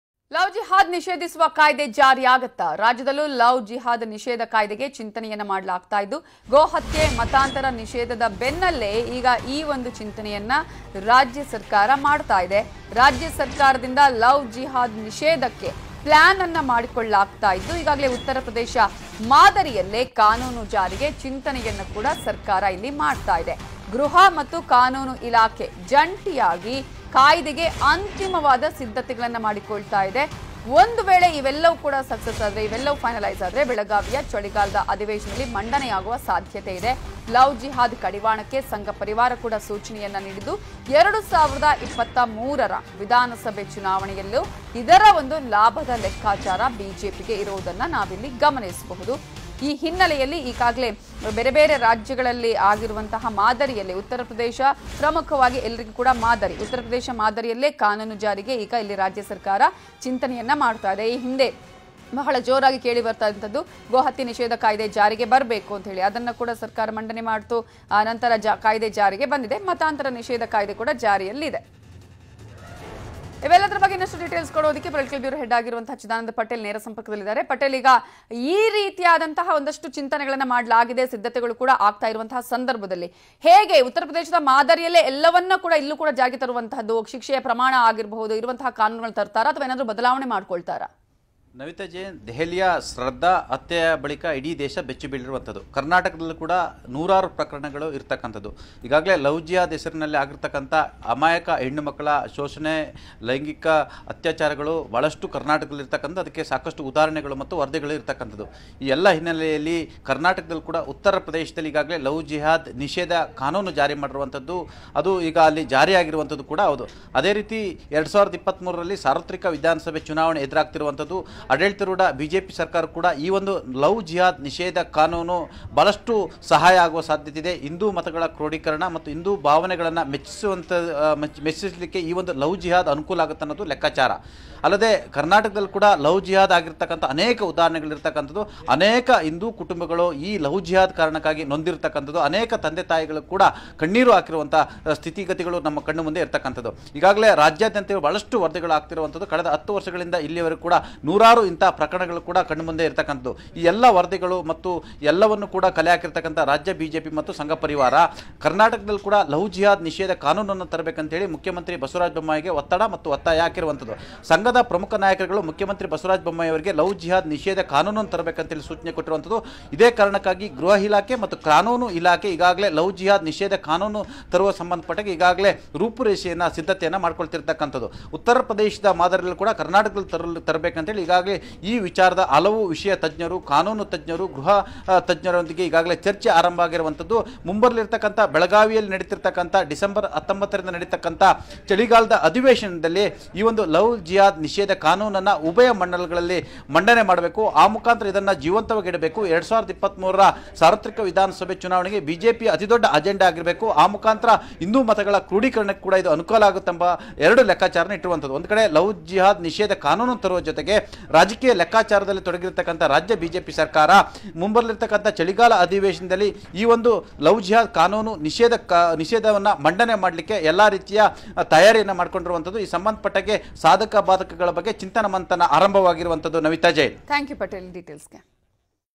Love Jihad | ರಾಜ್ಯದಲ್ಲೂ ಲವ್​ ಜಿಹಾದ್ ನಿಷೇಧ ಕಾಯ್ದೆಗೆ ಚಿಂತನೆ | News18 Kannada
Kannada News | ರಾಜ್ಯ ಸರ್ಕಾರದಿಂದ Love Jihad ನಿಷೇಧಕ್ಕೆ ಪ್ಲ್ಯಾನ್​. ಉತ್ತರ ಪ್ರದೇಶ ಮಾದರಿಯಲ್ಲೇ ಕಾನೂನು ಜಾರಿಗೆ ಚಿಂತನೆ.